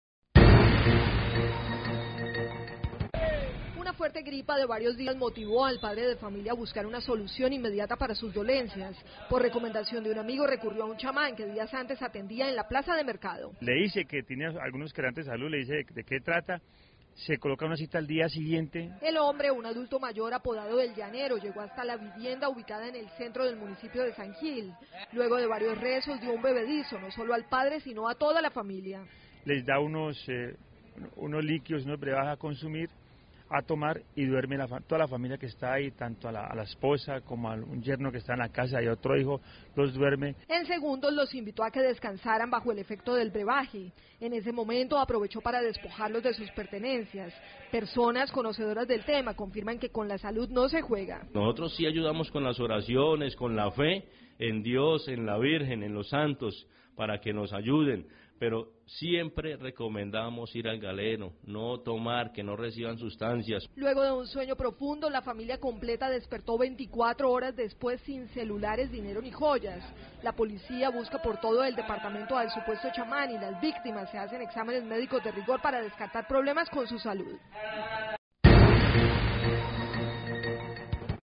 Chamán durmió una familia y robó la casa
Una familia que buscó los servicios de un chamán para curar una gripa terminó asaltada. Con un bebedizo el 'curandero' durmió a los esposos, sus dos hijas y su yerno y se llevó los objetos de valor de la casa.